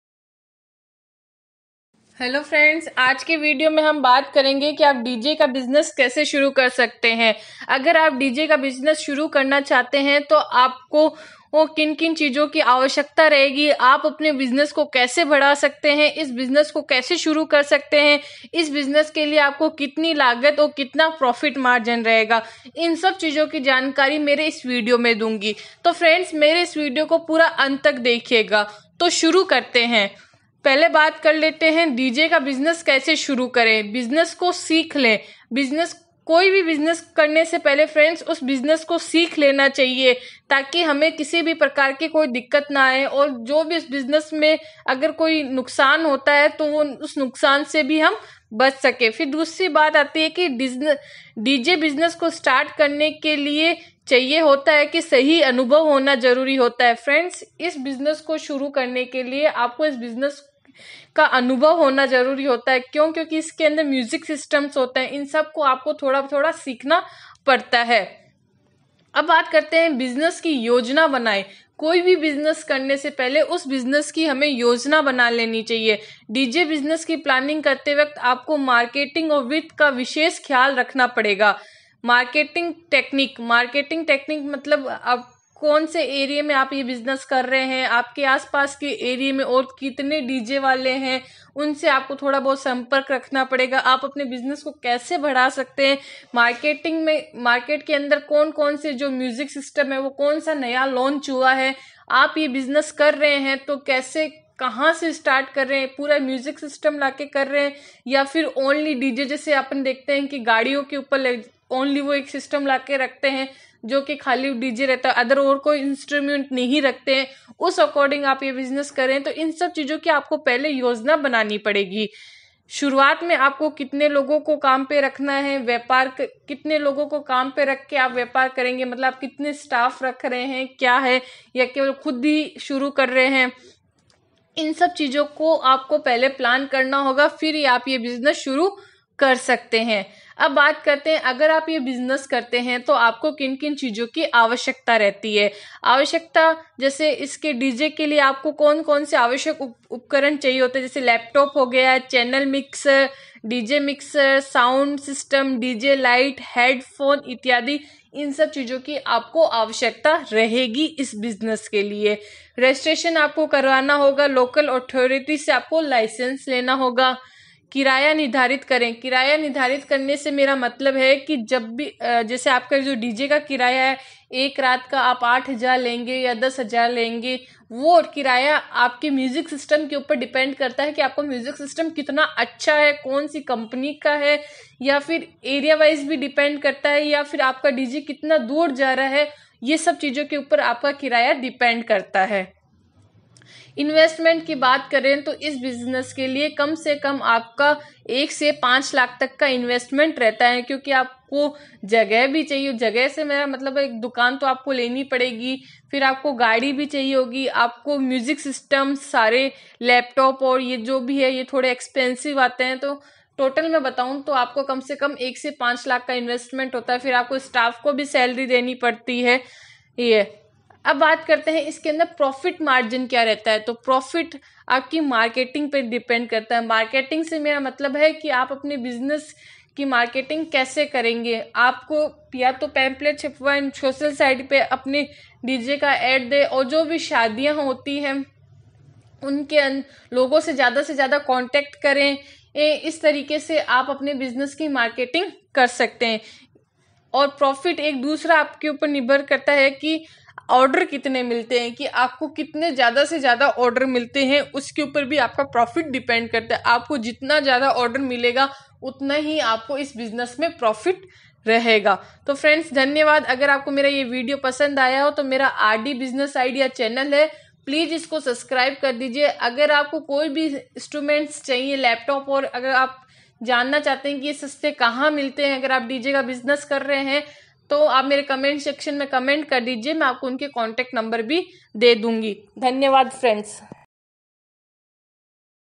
डीजे का बिज़नेस कैसे शुरू करे | dj business
दोस्तों आज के वीडियो में हम बात करेंगे की आप डीजे का बिज़नेस कैसे शुरू कर सकते है | अगर आप ये बिज़नेस करते है तो आपको किन - किन चीज़ो की आवश्यकता रहती है ,साथ ही इस बिज़नेस में आपको कितना प्रॉफिट मार्जन रहता है | अधिक जानकारी के लिए वीडियो अंत तक देखे |